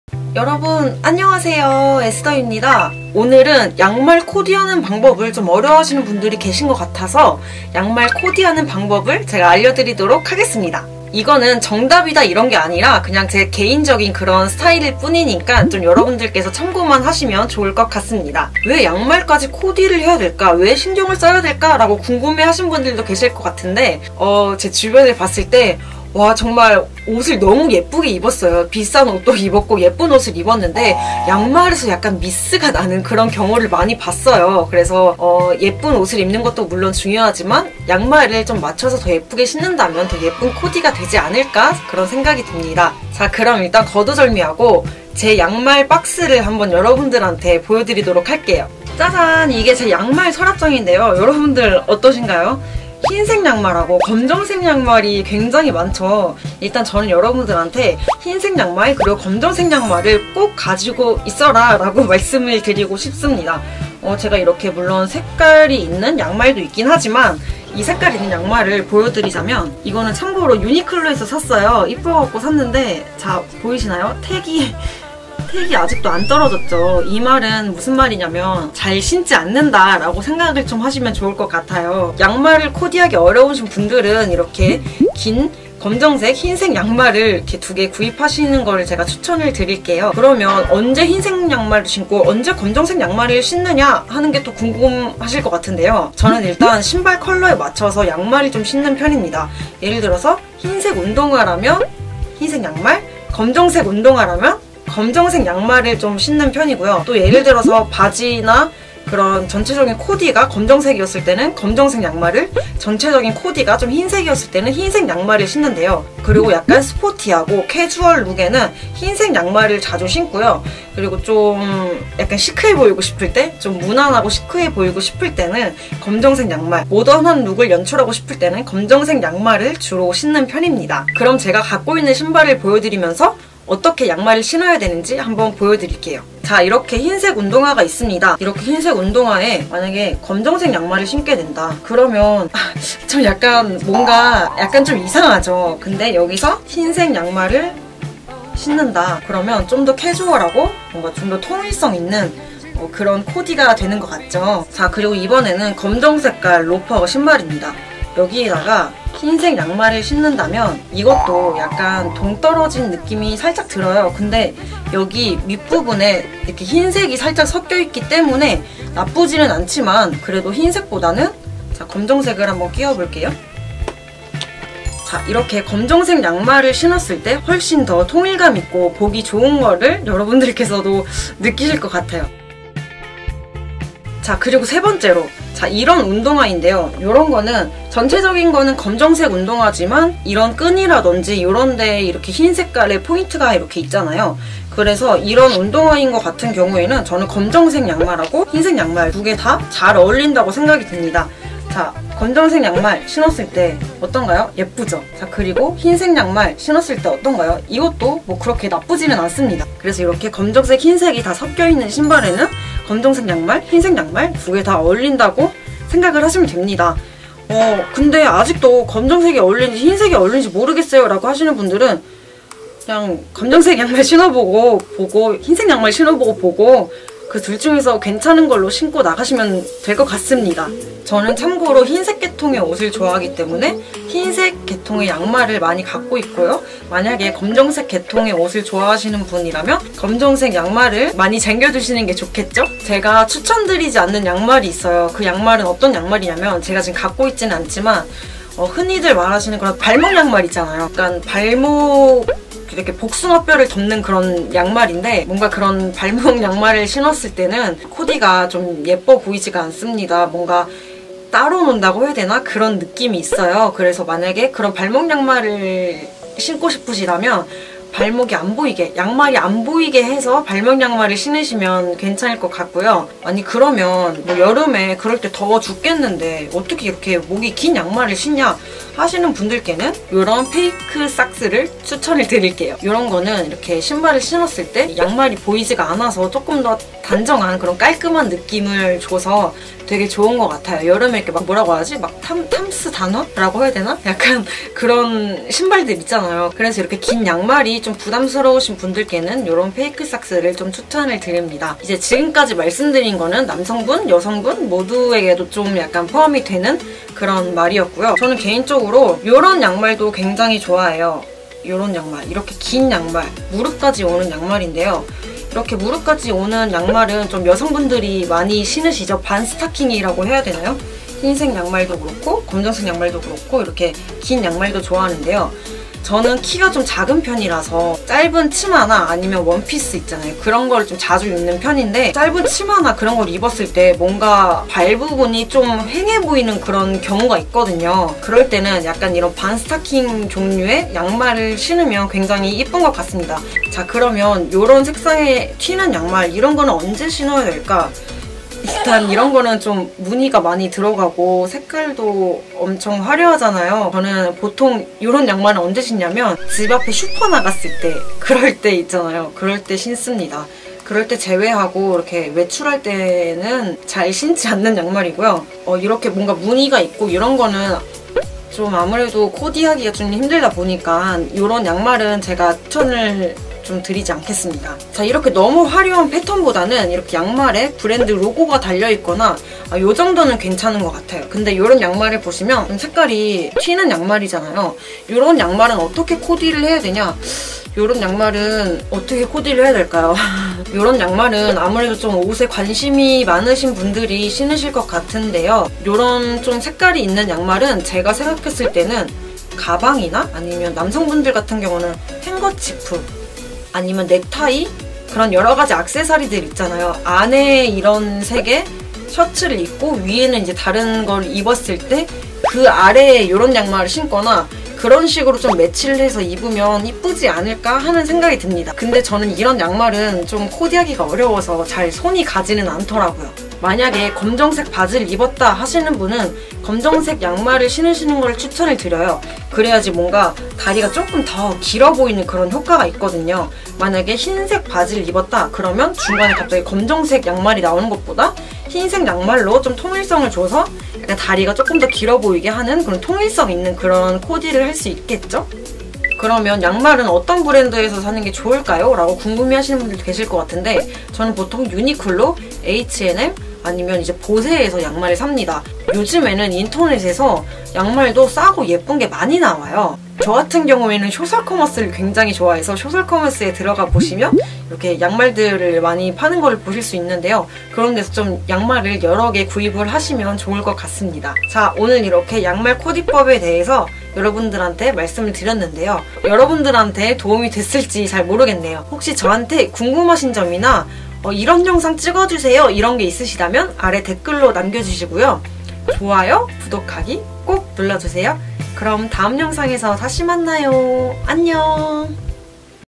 남,여 양말 코디하는 꿀팁 (색상,종류,구매처) socks fashion [박에스더]
안녕하세요 오늘은 의외로 많은분들이 궁금해하셨던
양말 코디하는 방법을 들고왔어요!
재미있게 봐주세요 !